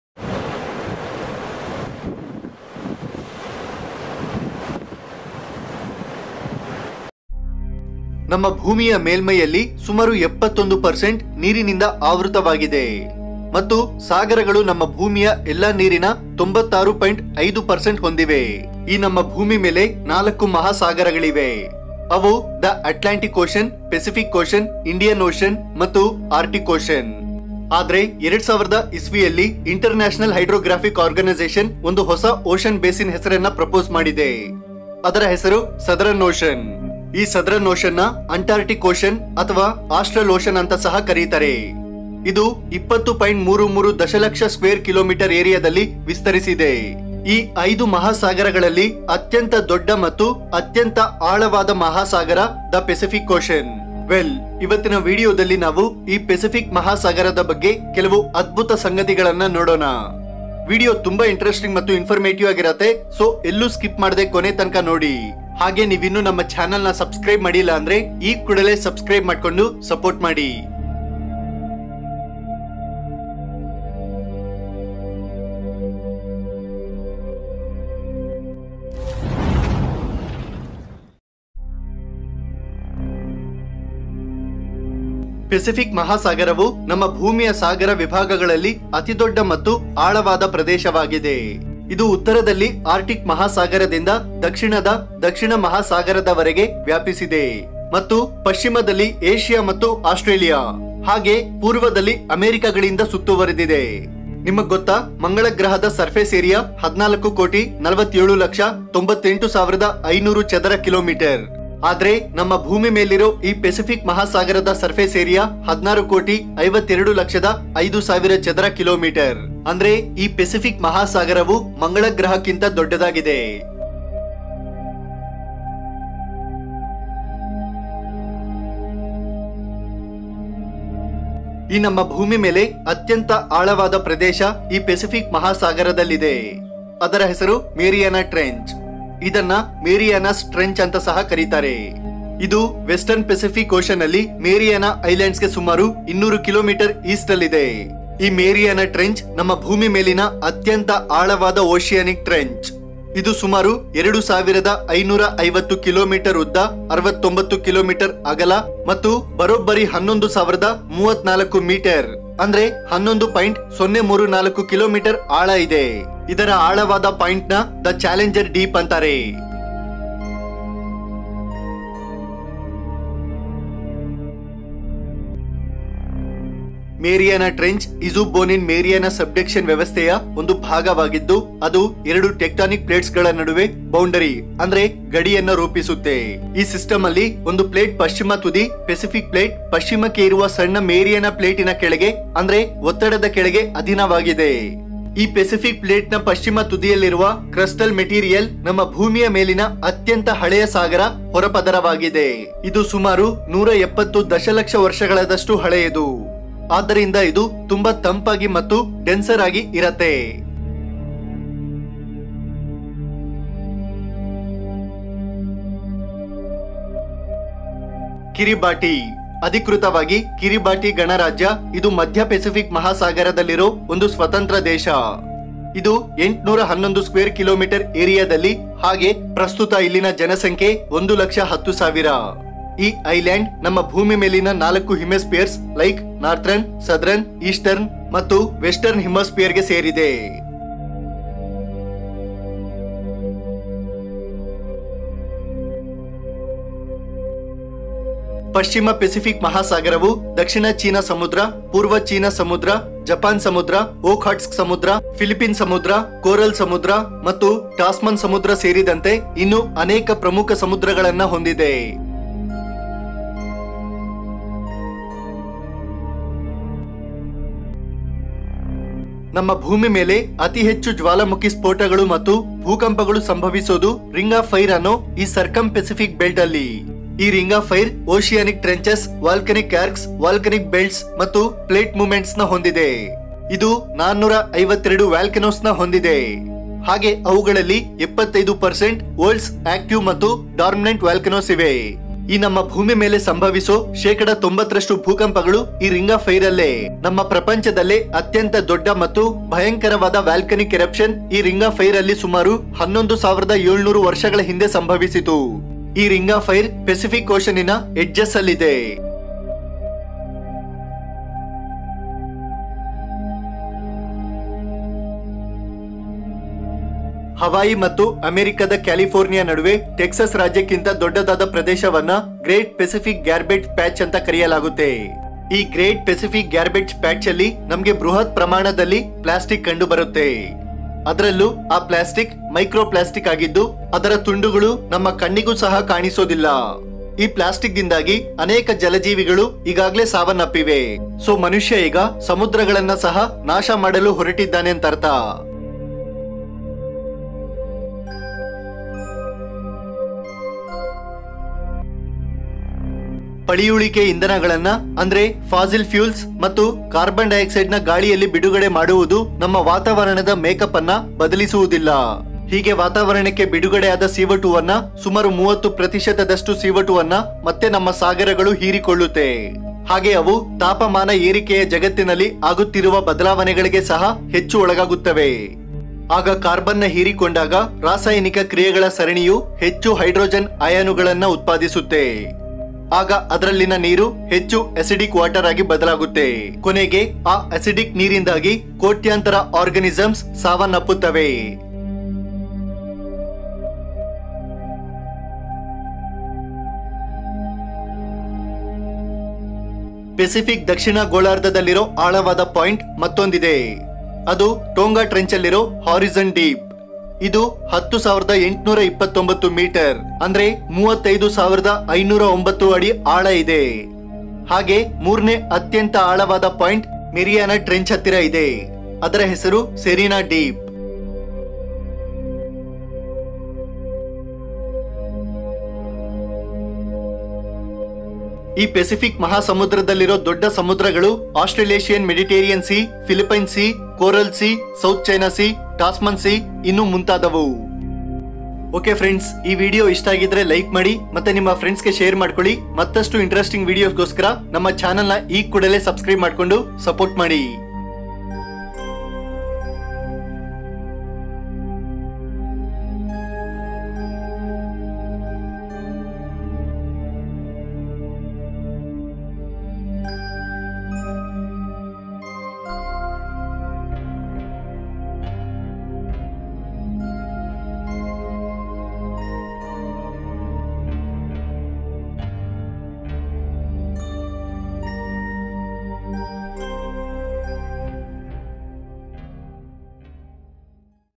Amazing and Interesting Facts About Pacific Ocean in Kannada
Hey guys, In this video I've explained about World's Largest And Deepest Ocean PACIFIC OCEAN In Kannada

👉Credit For Sources

👉Source 1: Google Images
👉 Source 2: PixaBay
👉Source 3: Free Stock Footage